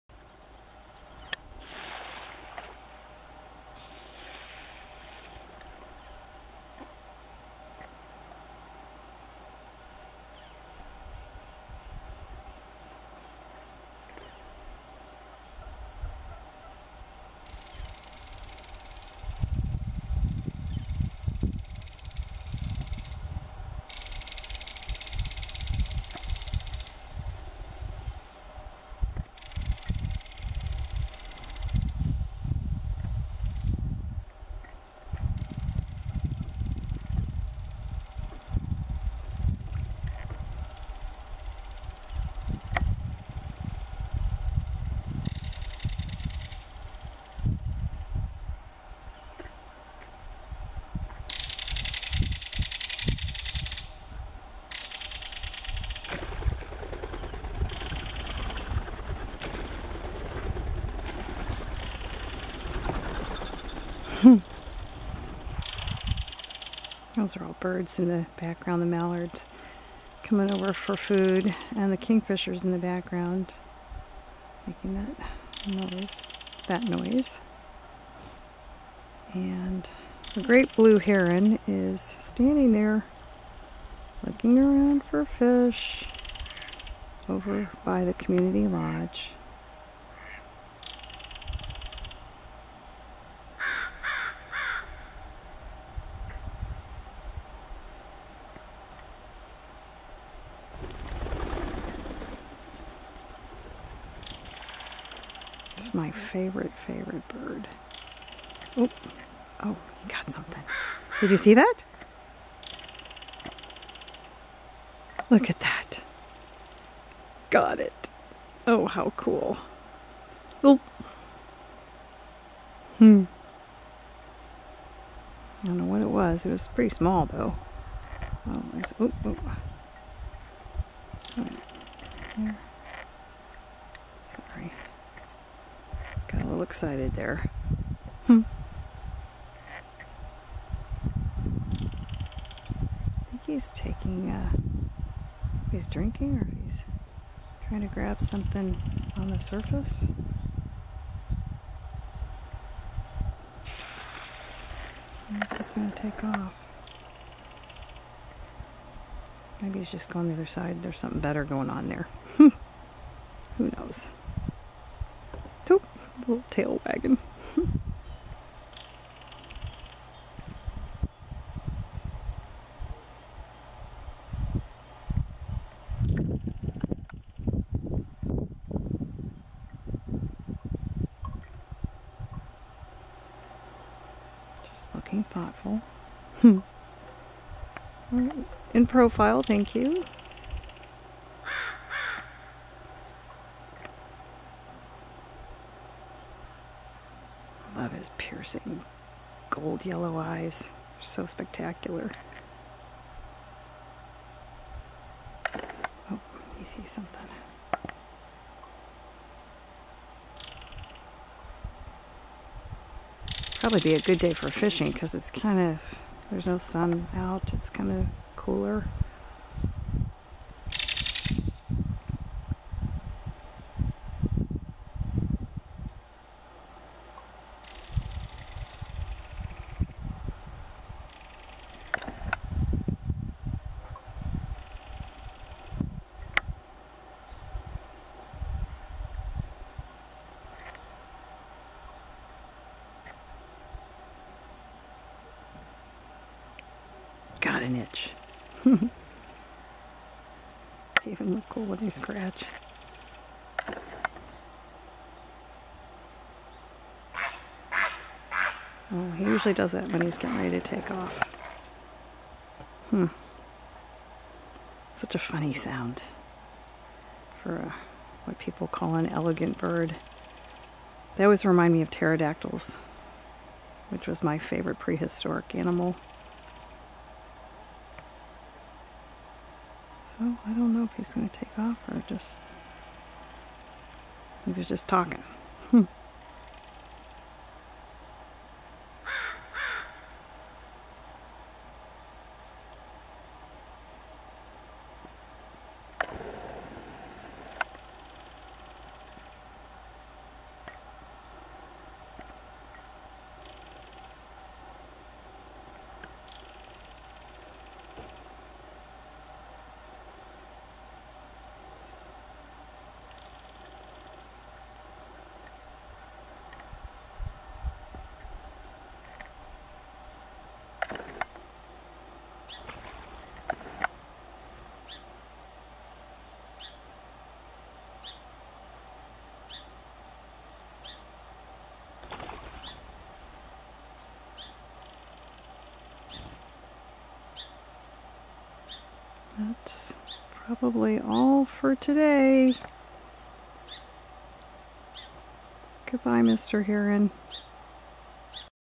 Great Blue Heron Grabs a Bite!
This was taken on  Fishhawk Lake on the main dock by the community lodge. An early evening on October 6, 2015.